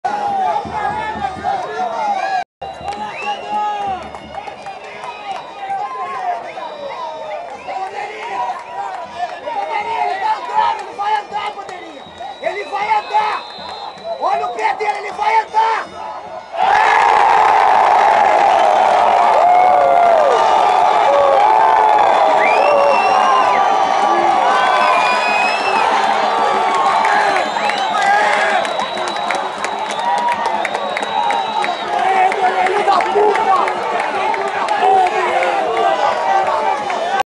Taubaté 6 x 1 Cal/Bariri - Gol Sandrinho
Primeiro gol da goleada Alvi Azul